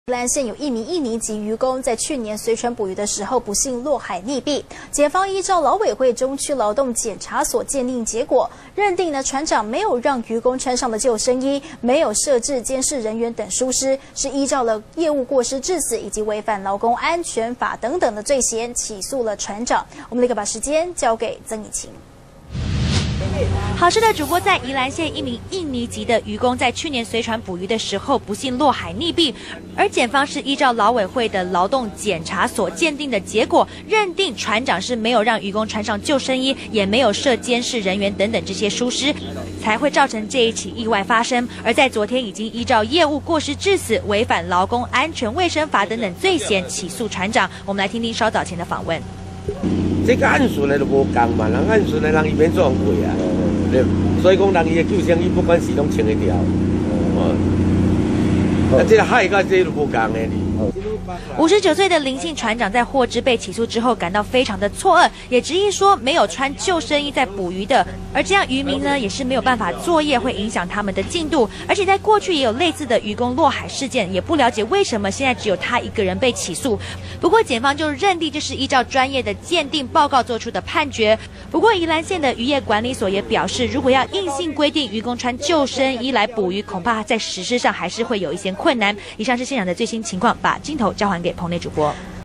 捕魚須穿救生衣？漁民嗆檢出海試試看
中天新聞》船長林耀通因船員未穿救生衣落海死亡遭訴，雖然法院尚未判決，但已引發漁民不滿，宜蘭縣延繩漁業協會理事長林日成表示，如判有罪「漁民會暴動！」認為檢察官應該親自出海了解現況；頭城區漁會總幹事鄭讚福則批起訴無理，以後「誰還敢出海捕魚？」